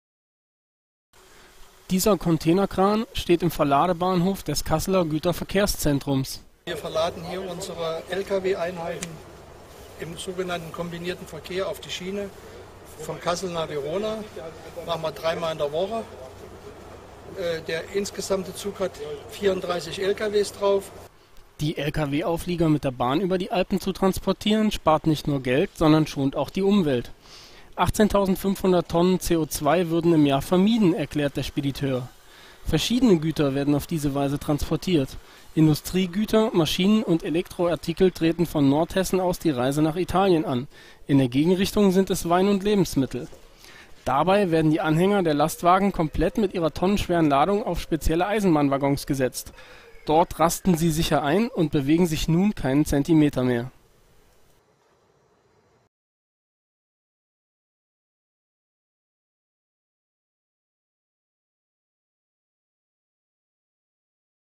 Kasseler Spedition Frölich schickt Frachtzug nach Italien
Kassel, 03.05.12: Die Spedition Fröhlich schickt dreimal in der Woche einen Frachtzug vom Kasseler GVZ aus ins italienische Verona. Dabei werden nicht nur Container, sondern komplette Lkw-Auflieger in der Nacht auf die Eisenbahnwaggons verladen.